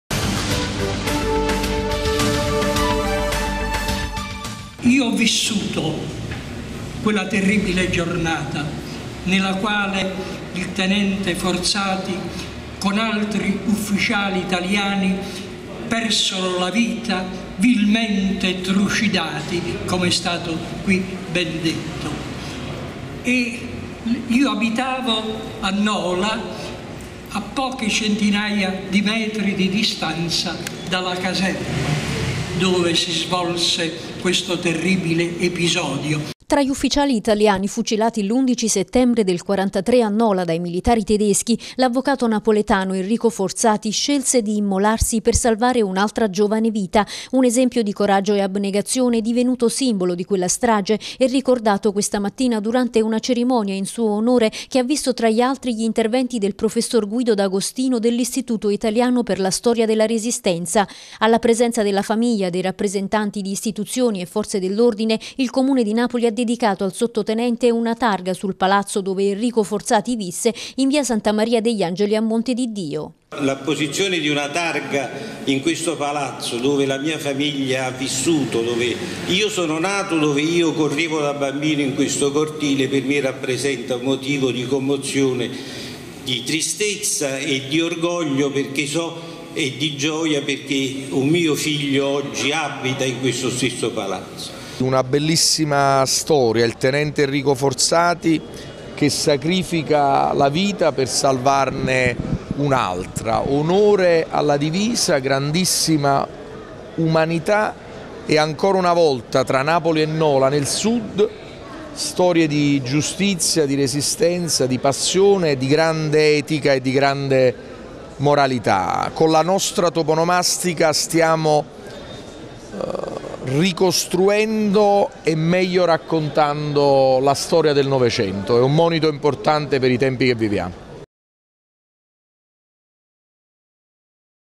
A Monte di Dio una targa per Enrico Forzati
Con una targa in piazza Santa Maria degli Angeli il Comune di Napoli ricorda Enrico Forzati, giovane sottotenente che sacrificò la sua vita nella rappresaglia tedesca del 1943 a Nola.